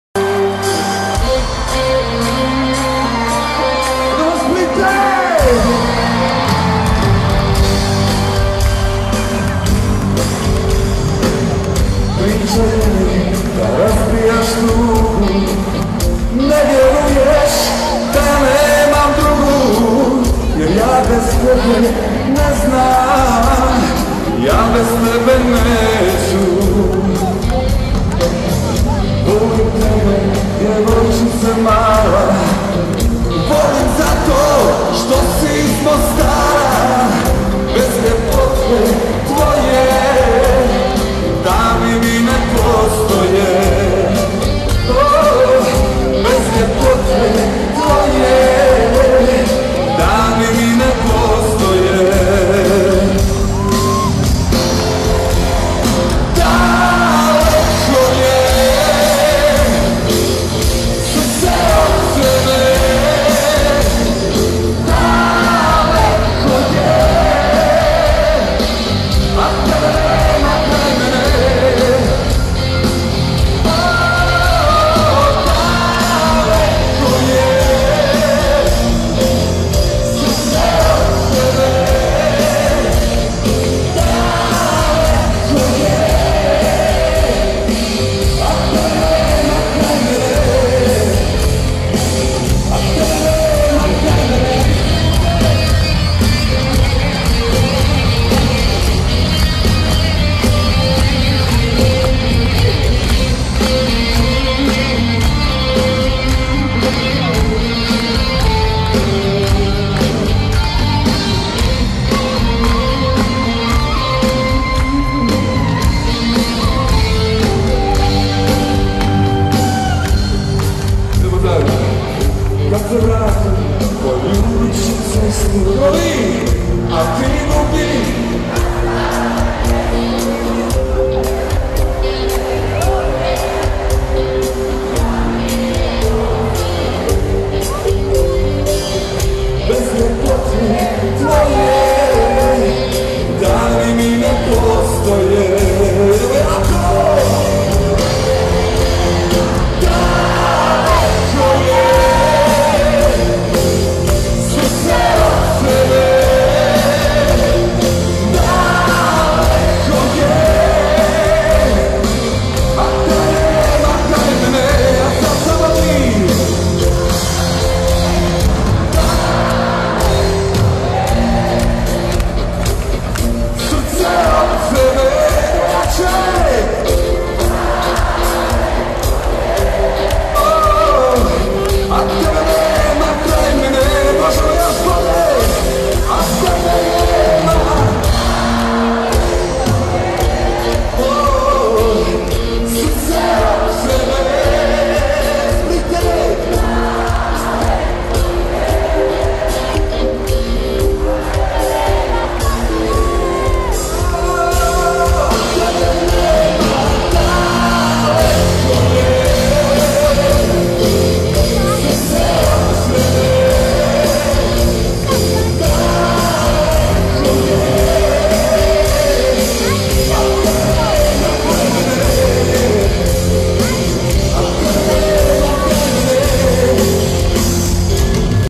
Humanitarni koncert u Spaladium Areni (2)
Humanitarni koncert u splitskoj Areni, nastup Marka Perkovića Thompsona i dr.